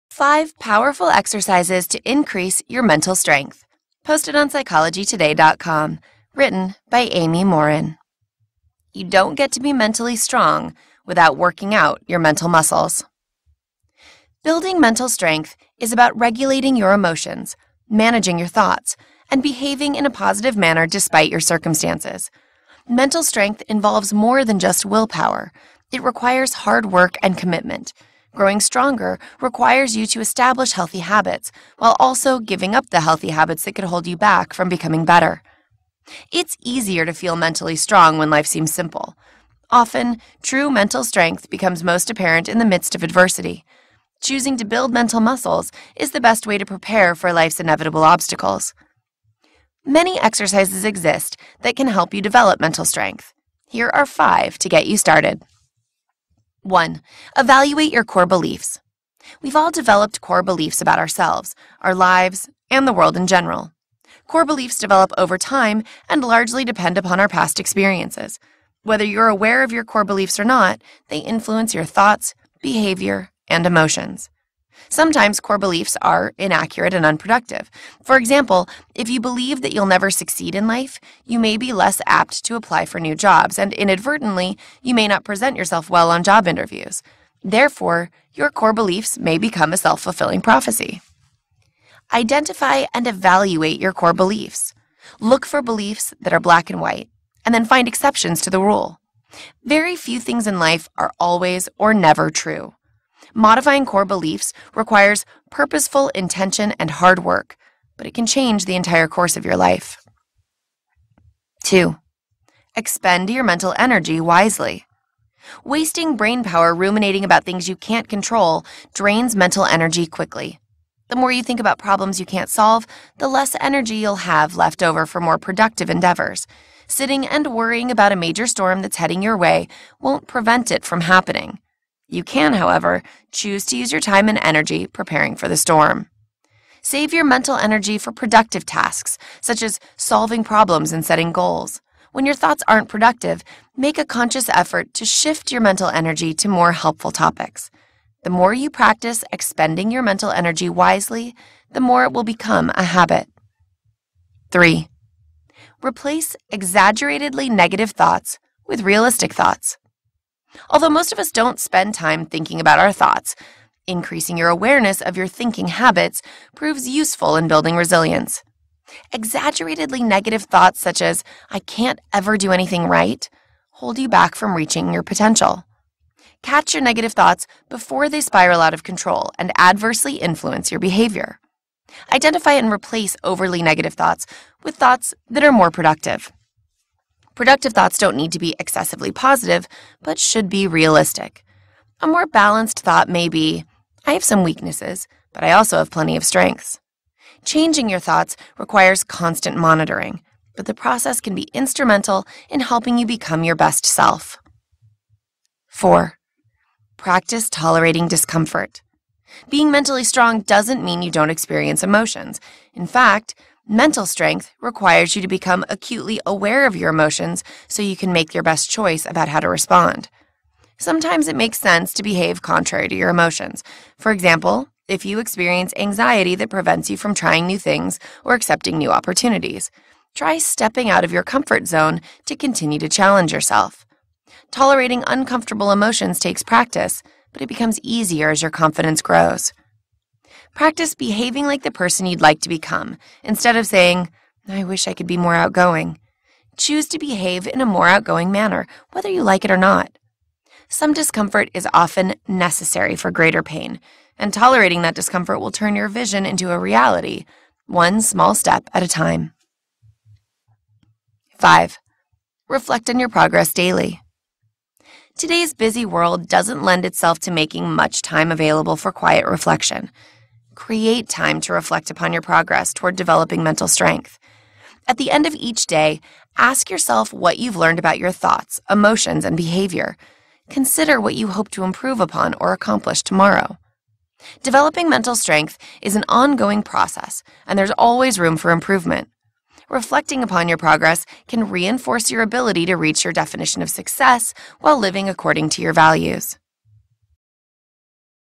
[ESL Tutorials] - 5 Powerful Exercises to Increase Your Mental Strength
You don't get to be mentally strong without working out your mental muscle
Building mental strength is about regulating your emotions, managing your thoughts, and behaving in a positive manner, despite your circumstances. Mental strength involves more than justwillpower; it requires hard work and commitment. Growing stronger requires you to establish healthy habits, while also giving up the unhealthy habits that could hold you back from becoming better.
It’s easier to feel mentally strong when life seems simple — often, true mental strength becomes most apparent in the midst of adversity. Choosing to build mental muscle is the best way to prepare for life’s inevitable obstacles.
Many exercises exist that can help you develop mental strength. Here are five that can get you started:
1. Evaluate Your Core Beliefs
We’ve all developed core beliefs about ourselves, our lives and the world in general. Core beliefs develop over time and largely depend upon our past experiences. Whether you’re aware of your core beliefs or not, they influence your thoughts, behavior and emotions.
Sometimes, core beliefs are inaccurate and unproductive. For example, if you believe that you’ll never succeed in life, you may be less apt to apply for new jobs — and inadvertently, you may not present yourself well on job interviews. Therefore, your core beliefs may become a self-fulfilling prophecy.
Identify and evaluate your core beliefs. Look for beliefs that are black and white, and then find exceptions to the rule. Very few things in life are “always” or “never” true. Modifying core beliefs requires purposeful intention and hard work, but it can change the entire course of your life.
2. Expend Your Mental Energy Wisely
Wasting brain power ruminating about things you can’t control drains mental energy quickly. The more you think about problems you can’t solve, the less energy you’ll have left over for more productive endeavors. Sitting and worrying about a major storm that’s headed your way won’t prevent it from happening. You can, however, choose to use your time and energy preparing for the storm.